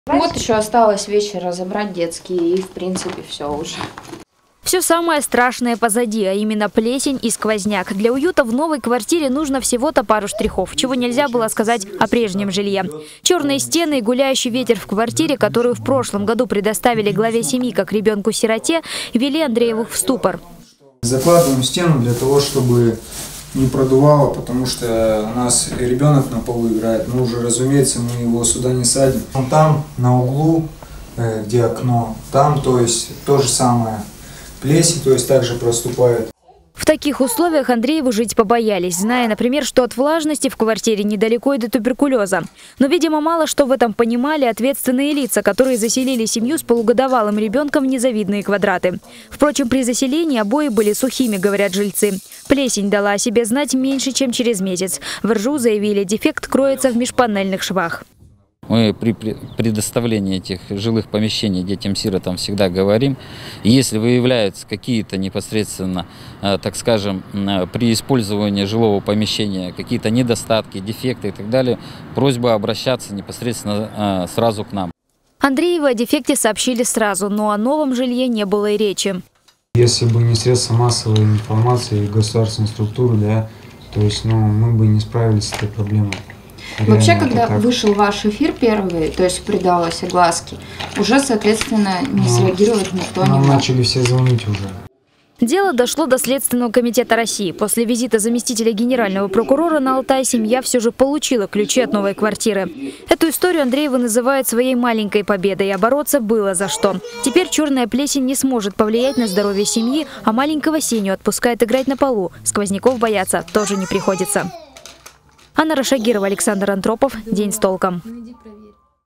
Как избавиться от плесени в квартире знает семья из Новоалтайска. Нужно сменить саму квартиру
Как избавиться от плесени в квартире знает семья из Новоалтайска. Нужно сменить саму квартиру. Мужчине из категории детей-сирот это далось совсем непросто. Минувшей зимой ему предоставили жилье не в надлежащем состоянии, и только недавно проблему удалось разрешить